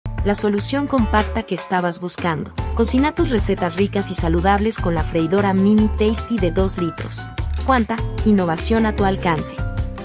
Freidora Electrica Minitasty 2L QTAF230 Quanta
La solución compacta que estabas buscando! 🍟🥗 Cocina tus recetas ricas y saludables con la Freidora Eléctrica Minitasty 2L de Quanta.

Para conocer los precios de nuestras freidoras, ingresá a nuestra web y seguí este

Pequeña en tamaño, pero grande en sabor y versatilidad. 🍽️

🚚 Envío gratuito en todas tus compras desde Gs. 150.000 al comprar en nuestra web.

📲 Consultá las zonas de cobertura en nuestra web.